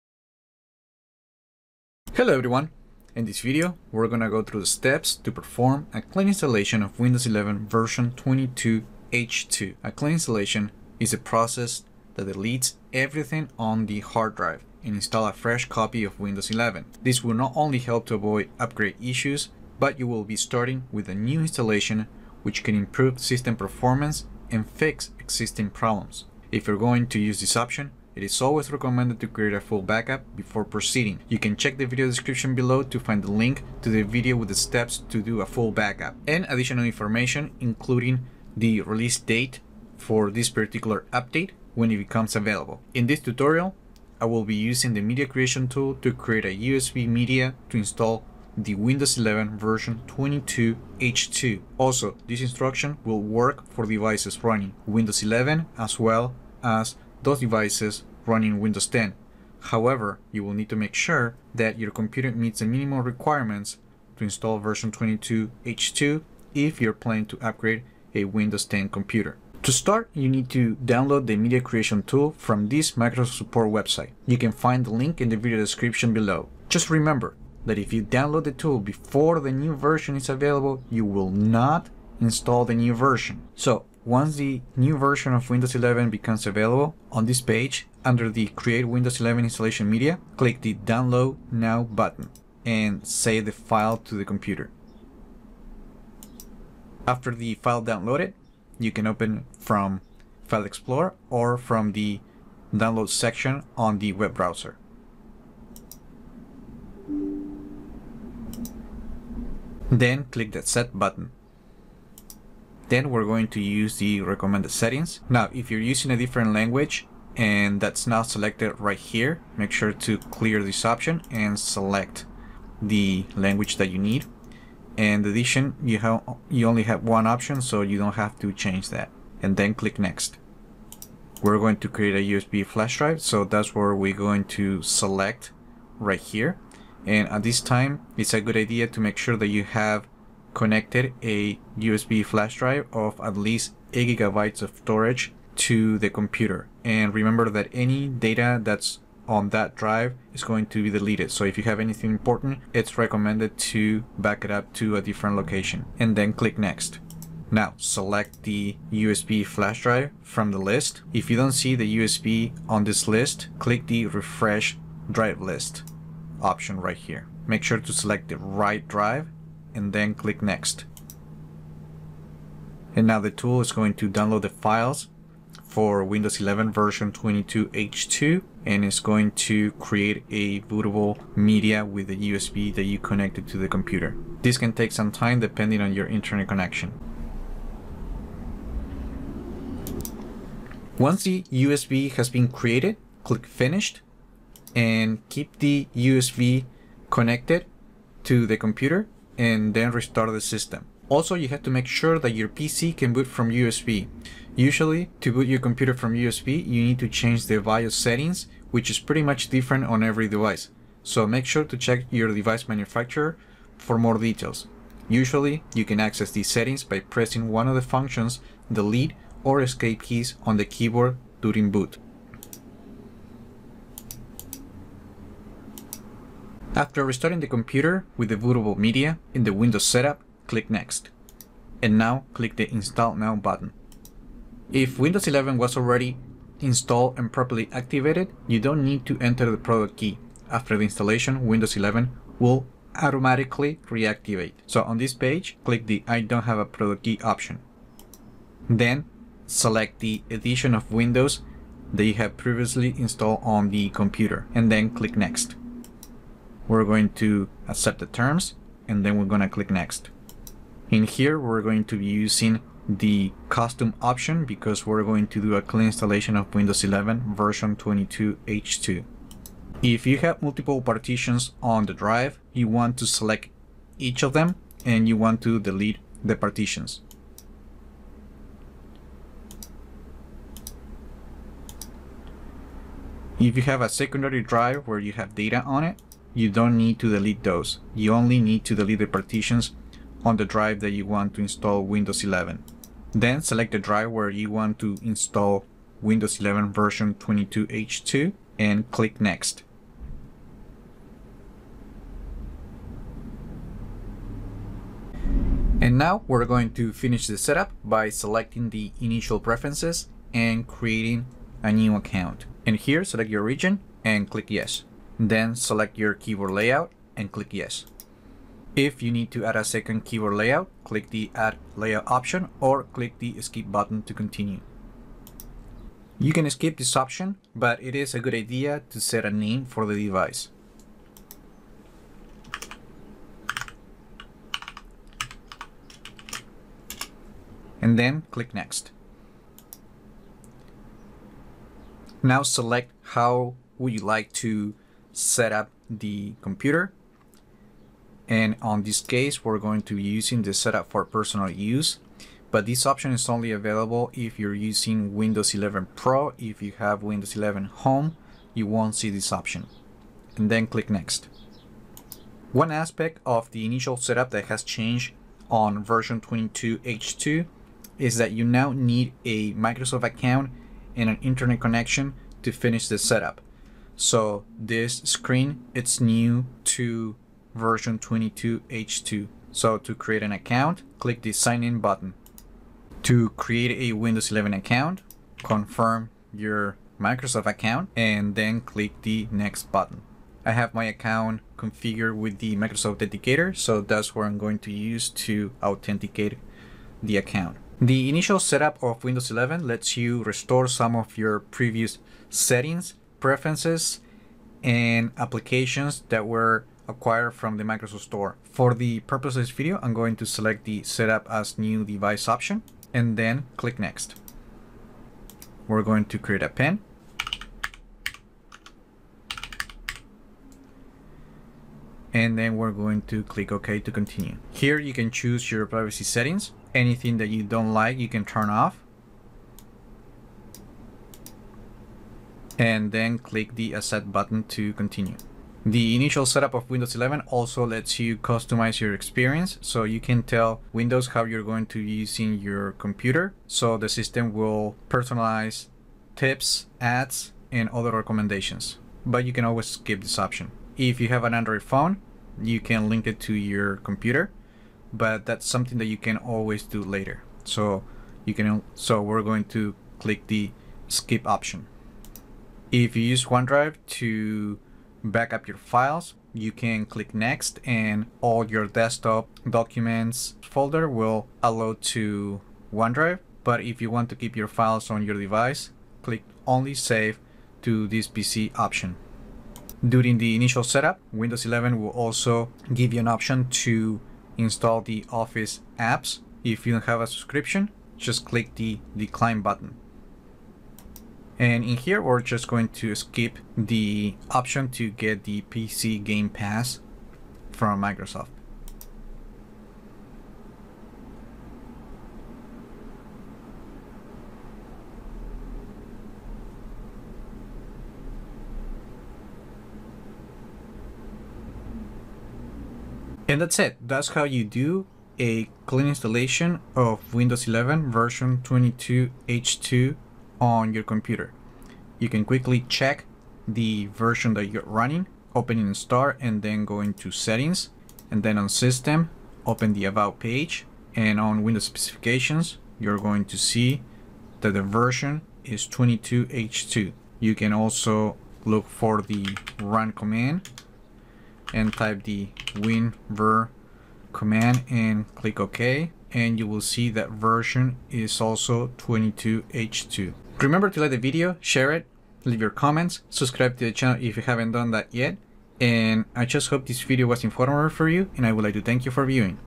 Windows 11 22H2: Clean install process from USB on SSD or HDD
Windows 11 tutorial on how to do a clean installation from USB on an SSD or HDD.
The video goes through all the steps from the creation of the bootable USB flash drive, to the clean installation of version 22h2, and completion of the OOBE.
🚨 Important: Windows 11 version 22H2 will release during the second half of 2022. If you try to install the update before it's officially available, you won't install the new version. These steps are meant for when the update is officially available. You have been warned.

Table of contents:
00:00 Intro
01:10 Create Windows 11 22H2 USB flash drive
03:47 Clean install Windows 11 22H2
05:05 Windows 11 22H2 OOBE setup
08:33 Check the version installed
09:10 Close